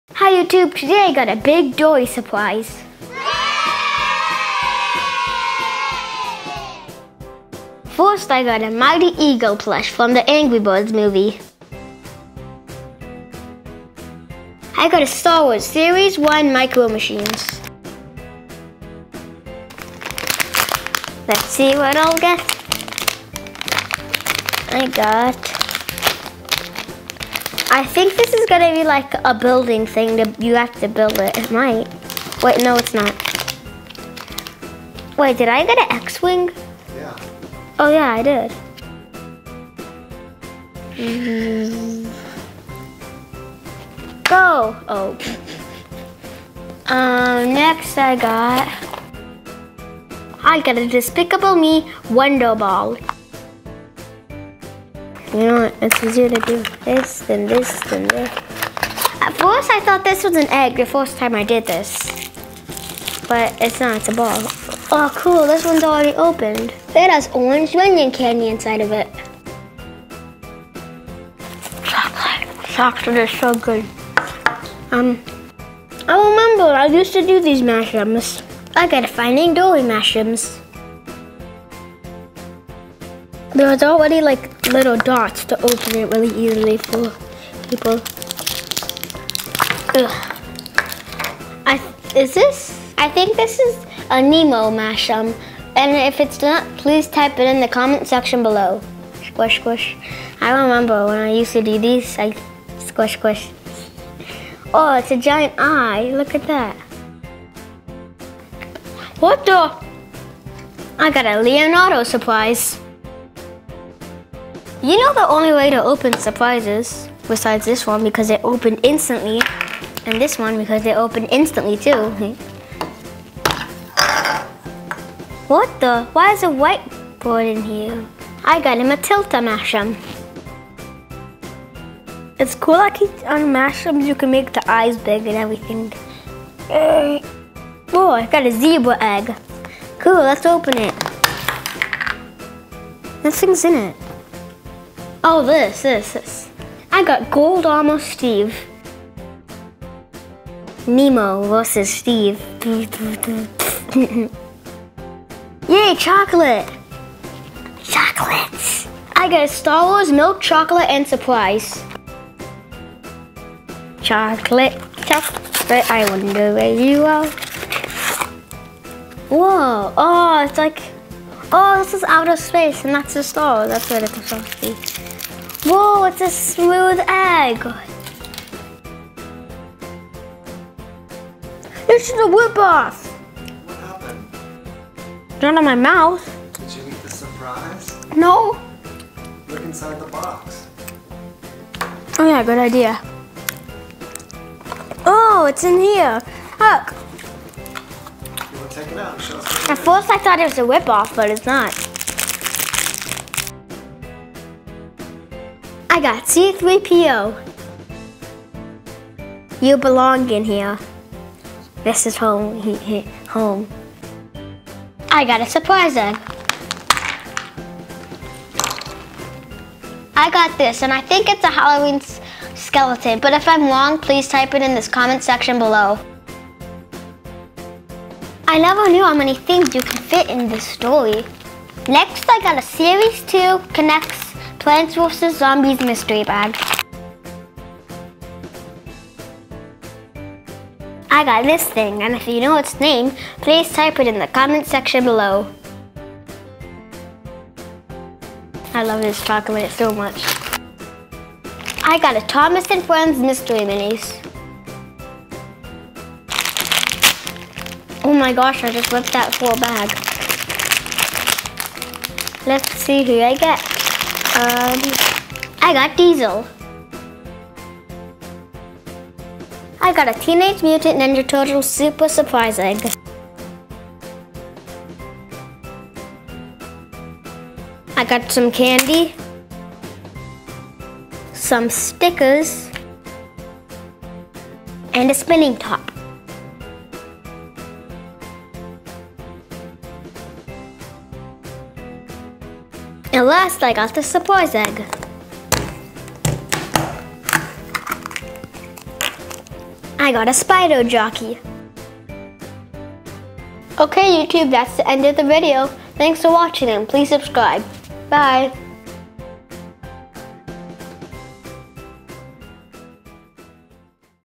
Finding Dory Surprise
William-Haik has a big finding dory surprise. Here is a list of some of the things that were inside: surprise eggs, a teenage mutant ninja turtles super surprise egg, a mighty eagle plush based on the angry birds movie, a series 1 finding dory mash'ems, a series 2 k'nex plants vs. zombies mystery bag, a leonardo surprise, a star wars milk chocolate and surprise and a series 1 star wars micro machines.